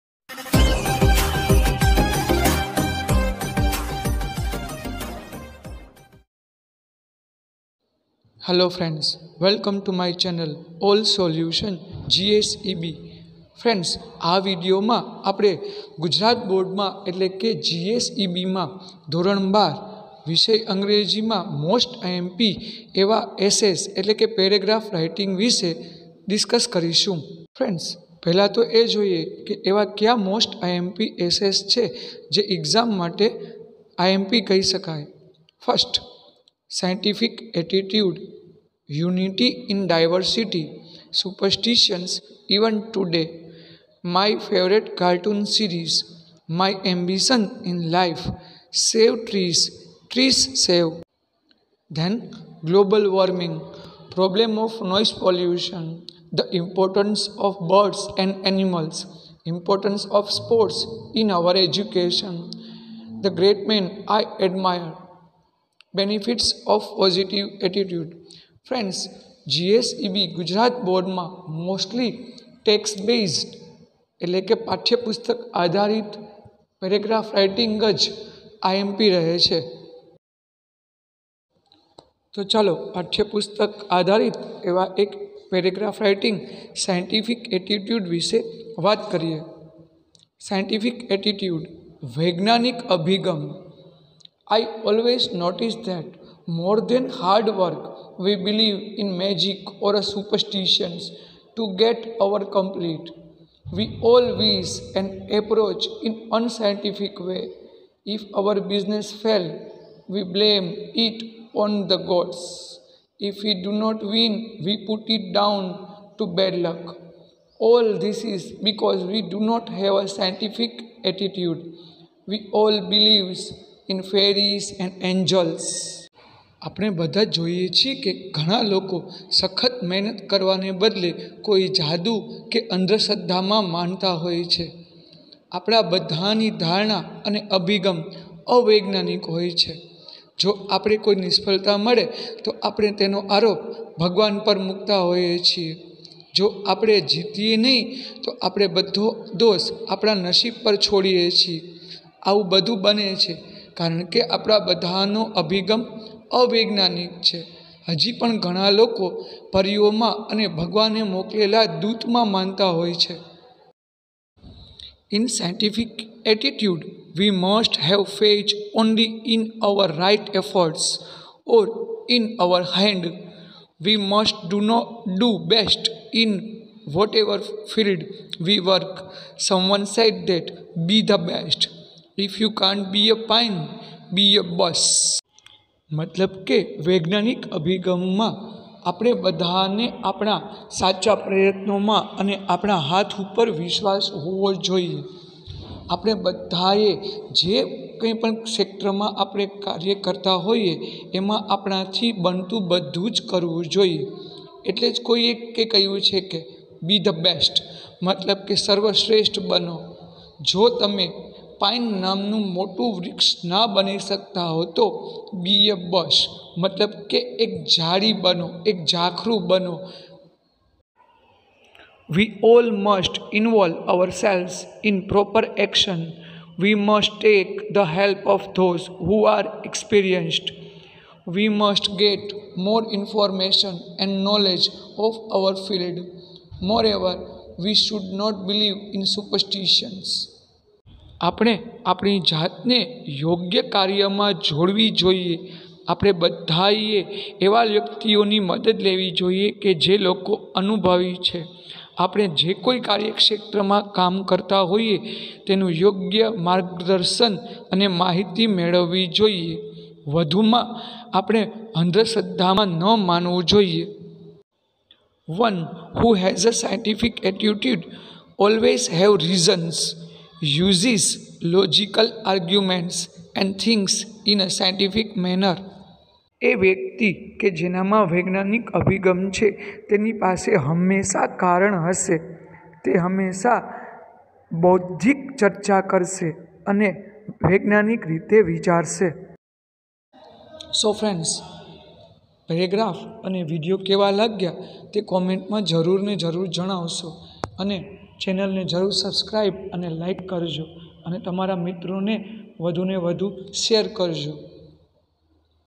Scientific Attitude|STD-12|GSEB|ALL SOLUTION GSEB|IMP PRAGRAPH WRITING |ESSAYS
Scientific Attitude|STD-12|GSEB|ALL SOLUTION GSEB|PARAGRAPH WRITING|Essays

     scientific attitude

            i always noticed that more than hard work, we believe in magic or a superstition to get our complete. we all wish and approach in unscientific way. if our business fail we blame it on the gods. if we do not win we put it down to bad luck. all this is because we do not have a scientific attitude. we all believe in fairies and angels.

        in scientific attitude, we must have faith only in our right efforts or in our hand. we must do best in whatever field we work. someone said that 'be the best if you can't be a pine, be a bush'.

     we all must involve ourselves in a proper action. we must take the help of those who are experienced. we must get more information and knowledge of our field. moreover we should not believe in superstitions.

     one who has a scientific attitude always have reasons, uses logical arguments and things in a scientific manner.